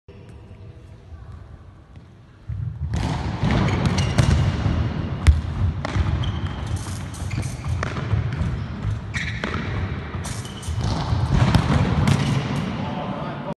Esercizio per portieri di futsal Pasculli
Tutte le protezioni e non solo le troverete nel sito

 Esercizio che inizia quando la palla entra in porta,quello è il momento che il portiere si gira dalla parte dove entra la palla per poi fare una spaccata nel lato opposto,si rialza e tocca il palo per fare una parata con la pallina da tennis alta per tre volte.Dopo aver finito avviene un tiro laterale con la palla "normale" prima di aver colpito uno scivolo
Si ringrazia Alberto Mancini per la concessione del video